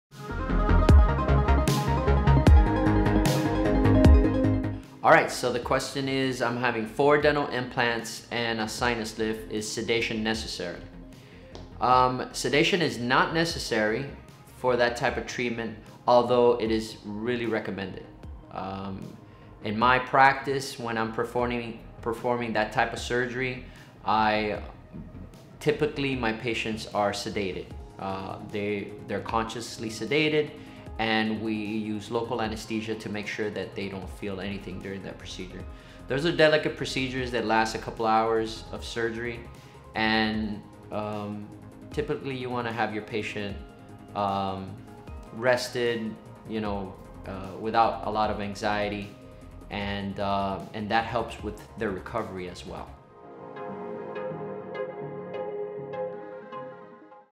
I am having four Dental Implants and a sinus lift; is sedation necessary?
ADDRESS:
William P. Lamas, DMD, MS - Periodontics & Dental Implants

Serving Miami For Over 20 Years.